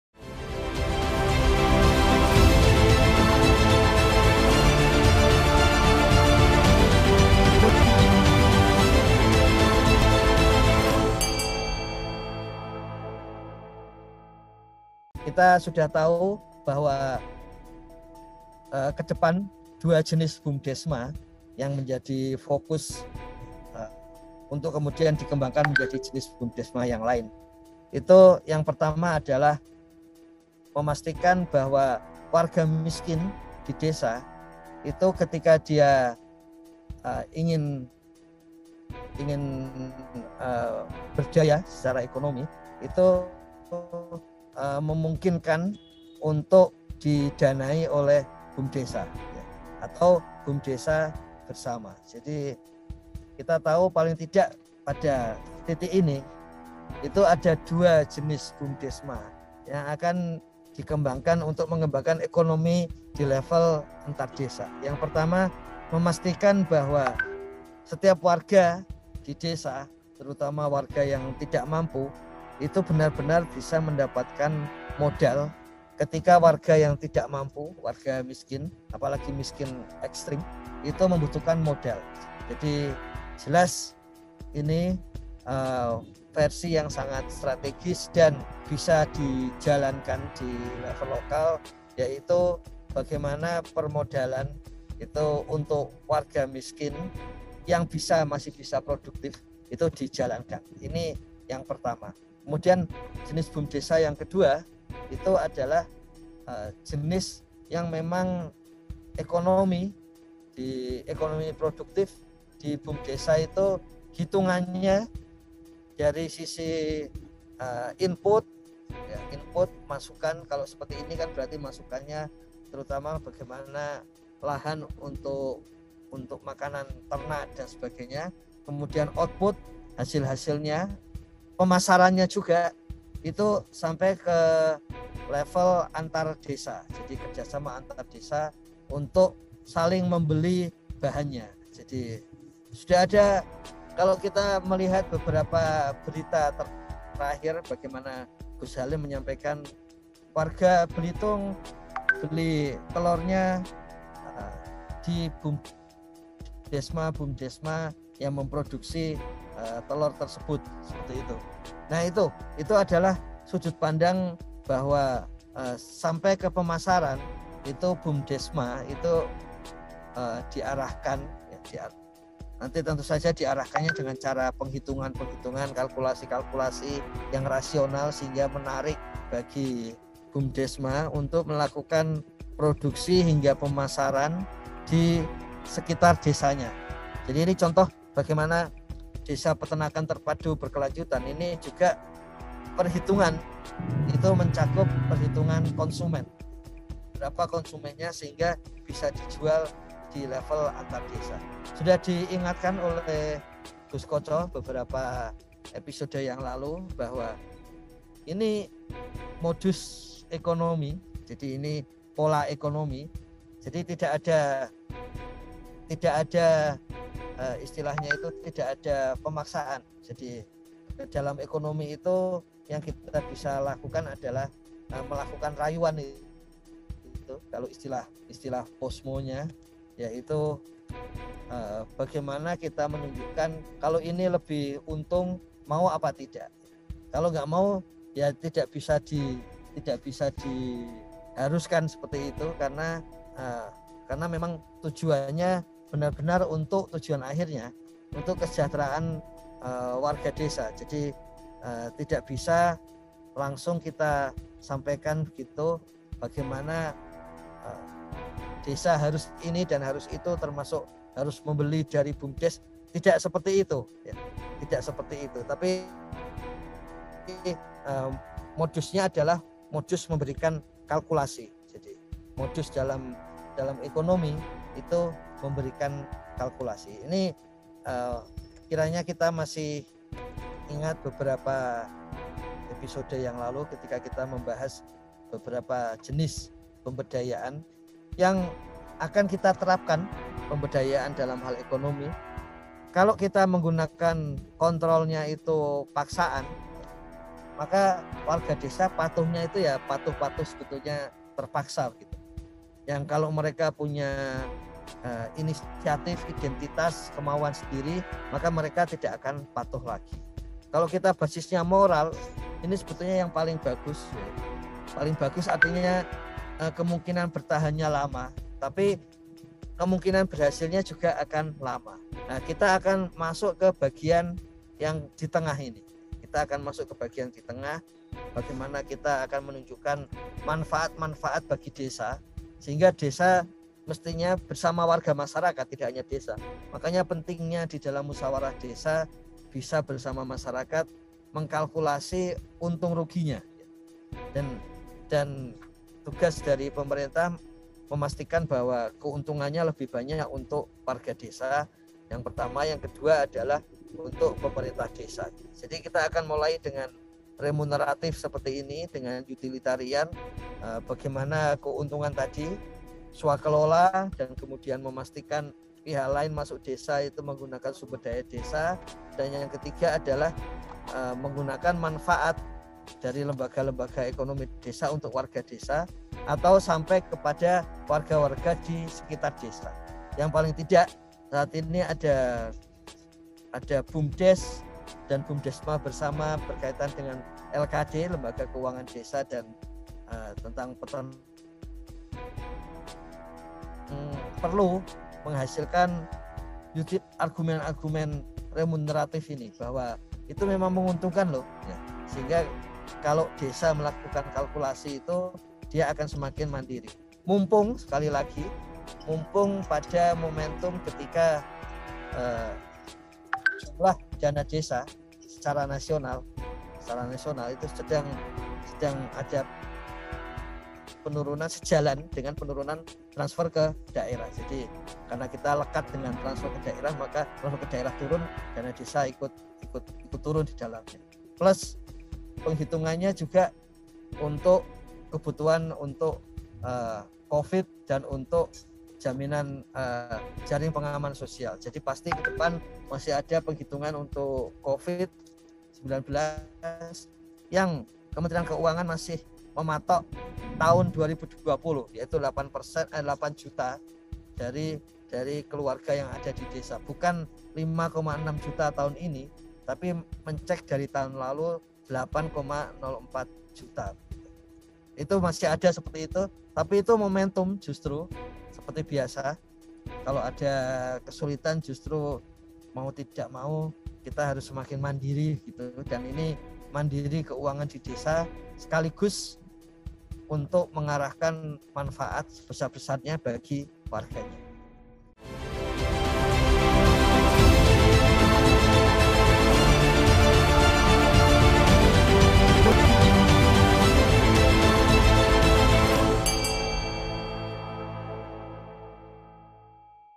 BUM Desa Bersama Akan Di Kembangkan Menjadi Bumdes Lain Selanjutnya
Prioritas penggunaan dana desa tahun 2021 ditetapkan dengan mengacu pada dua pertimbangan. Pertama ialah mengurangi dampak sosial dan ekonomi masyarakat serta terhambatnya pembangunan desa dan yang ke dua ialah menghadapi ancaman yang membahayakan sistem perekonomian negara dan/atau stabilitas sistem keuangan perlu melaksanakan kebijakan keuangan negara.